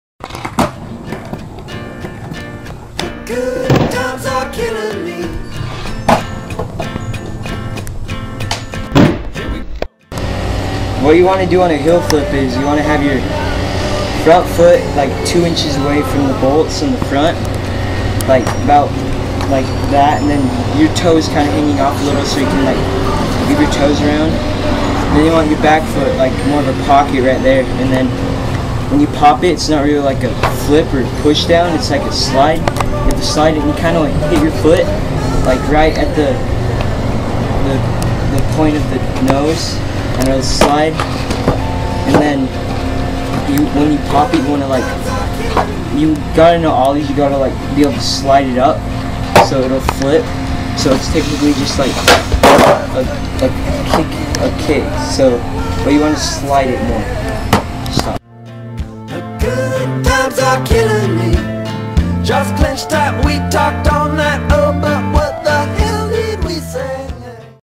Trick Tips: How To Heel Flip.wmv
How to heel flip explained and with stetson jeppson, filmed by josh stucki